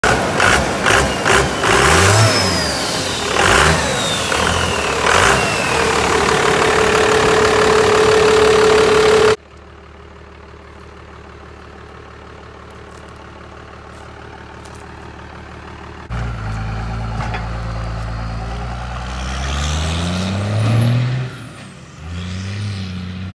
12 valve Cummins first drive after fass install
95 12 valve Cummins stock as far as we know was having major fuel issues seemed to be getting air in the lines cause white smoke and random stalls with power loss. Installed a fass 140 and a beans sump and it runs great now this is first idle and drive in almost a year when it started having problems